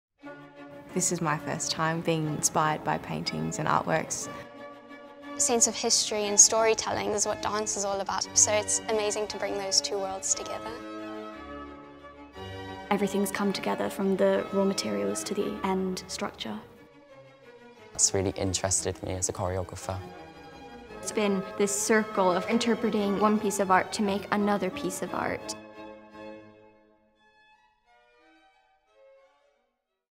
English National Ballet School | Young Choreographers 2021 trailer
Join us from 7pm (BST) on Wednesday 23 June when we premiere Young Choreographers 2021.

Filmed at The Wallace Collection this selection of 10 original dance works, choreographed and performed by our students, is the result of a unique collaboration between English National Ballet School, The Wallace Collection, the London College of Fashion and the Royal College of Music.